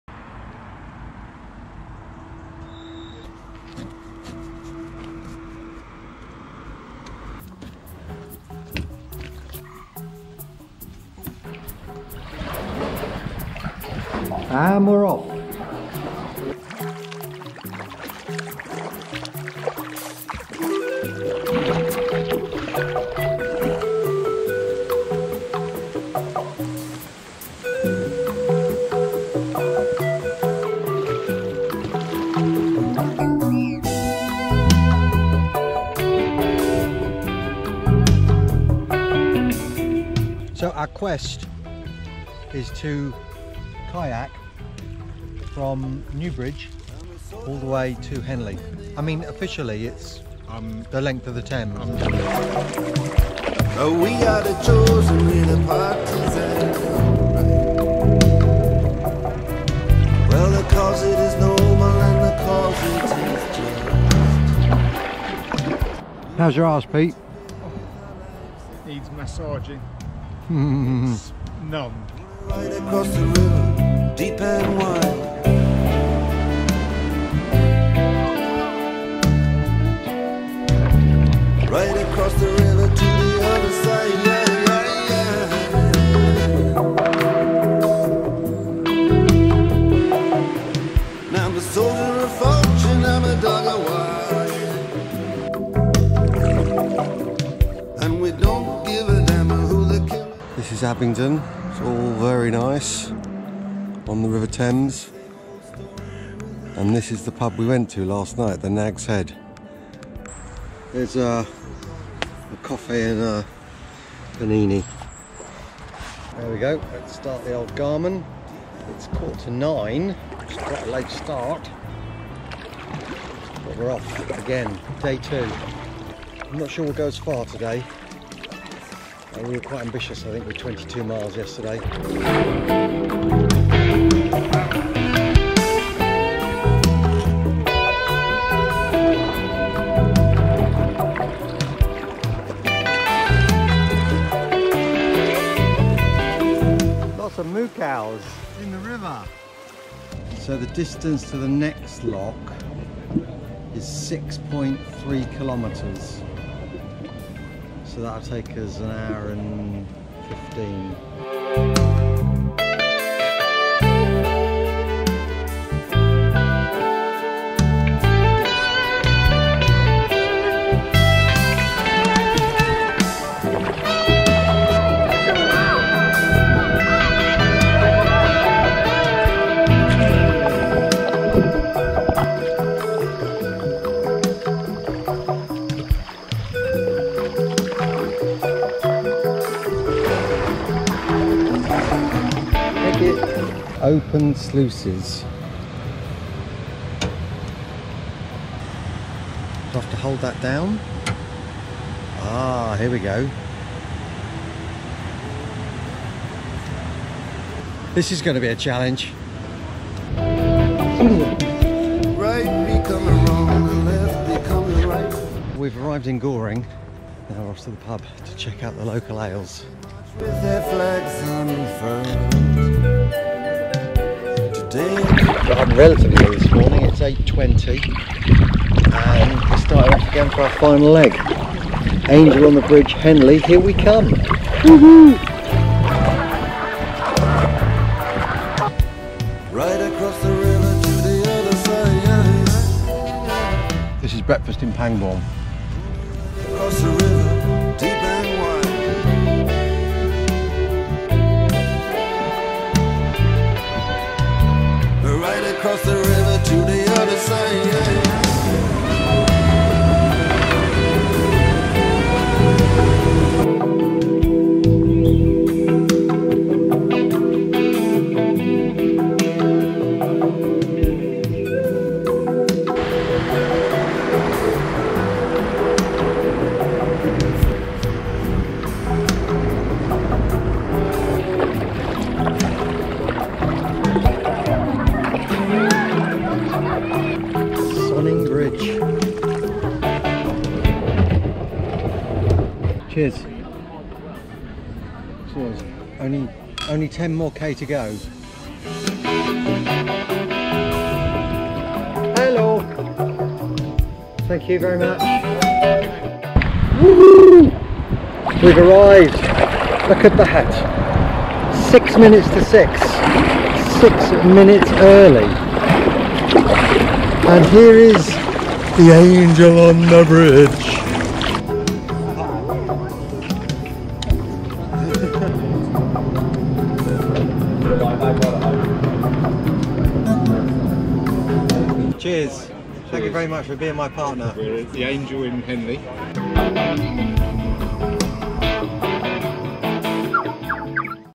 Kayaking the River Thames (short)
From Newbridge to Henley, the exploits of 2 blokes kayaking 22 miles a day down the River Thames